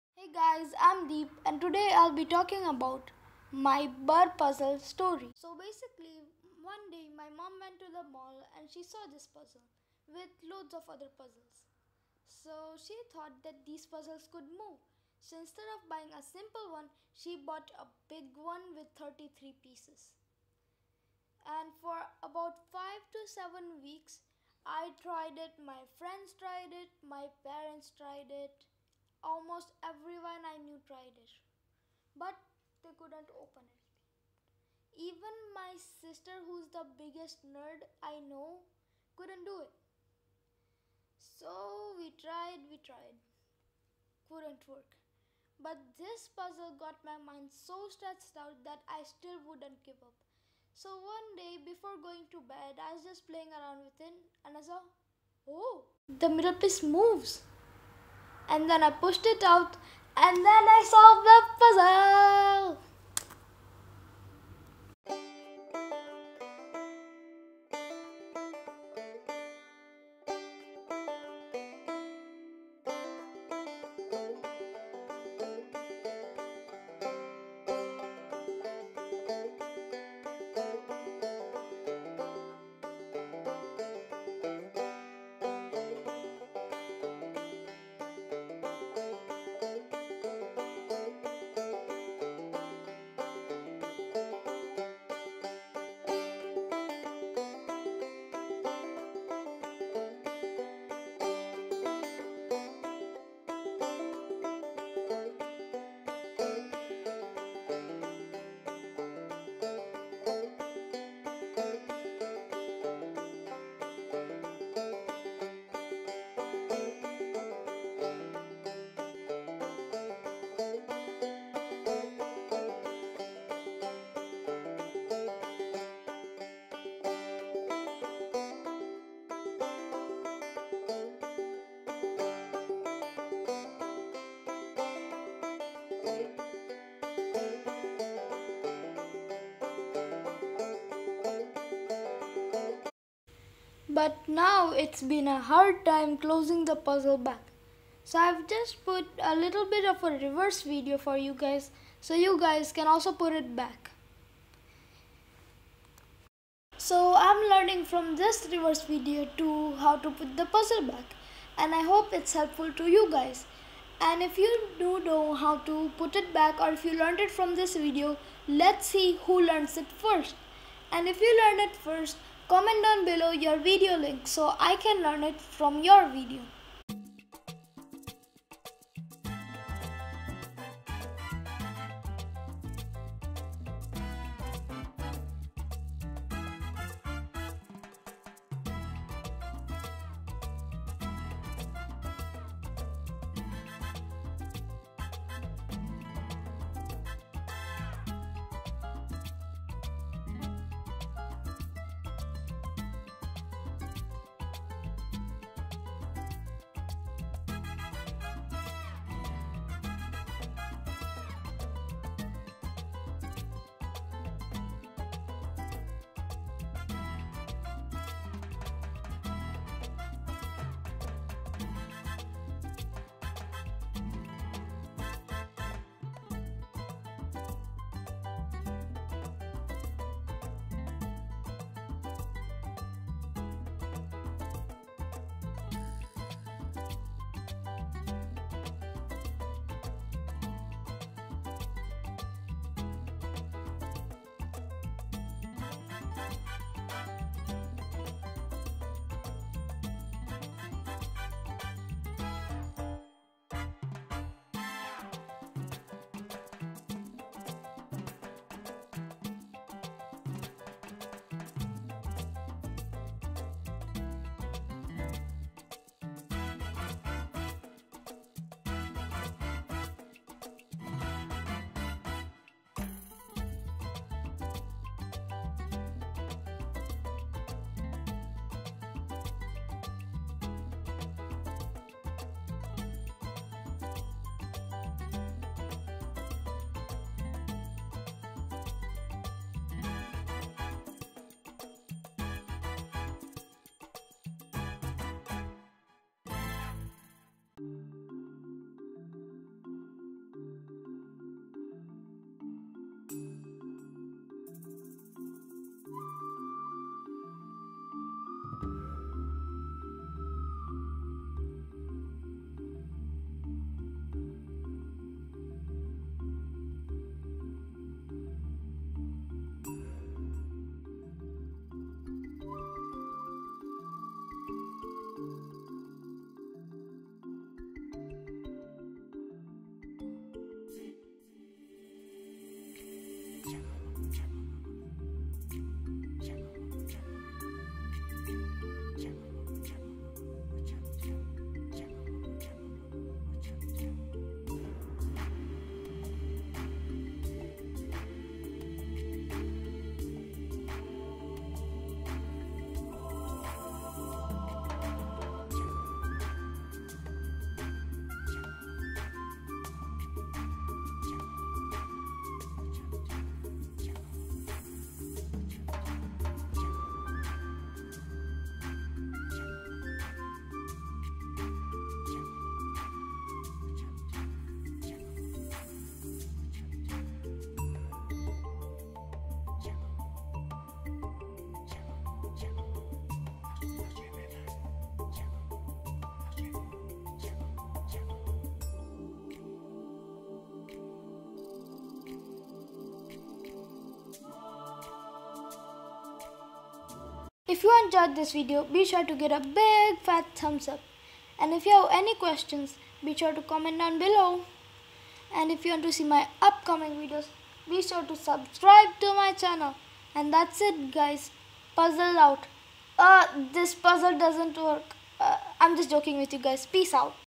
burr puzzle impossible wooden puzzle
burr puzzle impossible wooden puzzle impossible puzzle
this is 3d puzzle which was very hard for me to solve. but it is fun for family. I can say family friendly toy. I was searching for the solution for long time and finally solved it. I mean I learned how to open it. so I made here a tutorial video. it was a brain workout lol. 33 pieces are very difficult to manage for how to close
burrpuzzle with DJ Patel